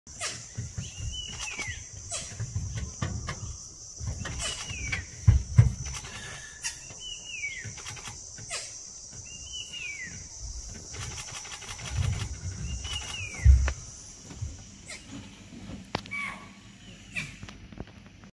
The Sri Lankan leopard🐆 | Sri Lanka Panthera pardus kotiya
The Sri Lankan leopard 🐆 (Panthera pardus kotiya) is a magnificent and highly endangered big cat species found in the Wilpattu National Park 🌳🐾, located in Sri Lanka 🇱🇰. Renowned for its striking beauty and elusive nature, the Sri Lankan leopard is the apex predator of the park's diverse ecosystem 🌿🐇🐿️🦌🐃.